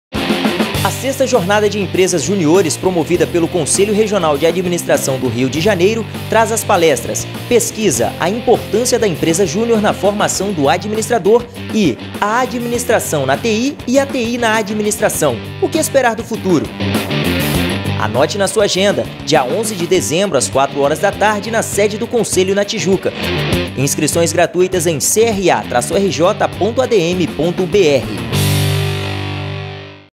VI Jornada de Empresas Juniores - 11 de dezembro de 2014
O Conselho Regional de Administração do Rio de Janeiro promoverá a VI Jornada de Empresas Juniores, no próximo dia 11 de dezembro, a partir das 16h, no Auditório Adm. Gilda Nunes, sede do Conselho.